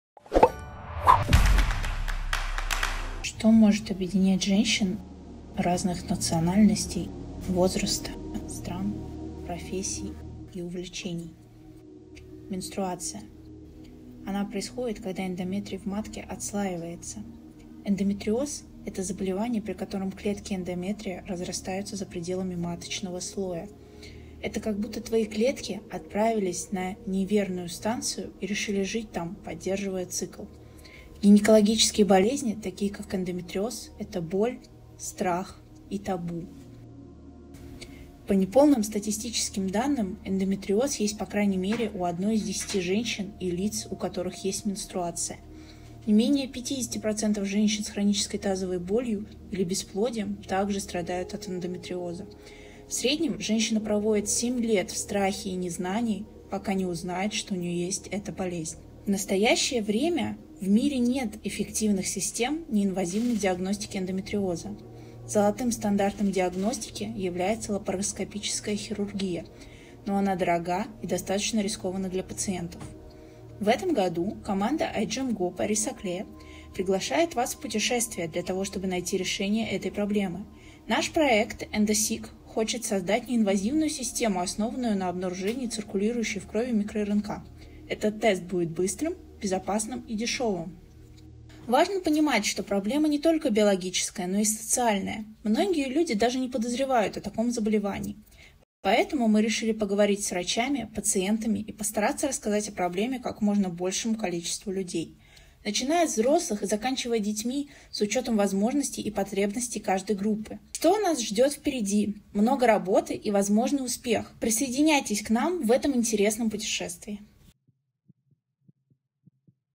GO Paris Saclay EndoSeek, detecting endometriosis 2021 Project Promotion (Russian)
Title: EndoSeek, detecting endometriosis

Description: iGEM GO Paris-Saclay 2021 is pleased to announce the project EndoSeek. It concerns the development of a new method of diagnosis of endometriosis. This disease affects 1 woman out of 10 causing painful symptoms for the patients: painful menstruation, infertility... Current diagnostic methods are costly and invasive. We propose to target de-regulated miRNAs after a blood test and to reveal them via a Crispr-Cas cascade. Join us on this journey!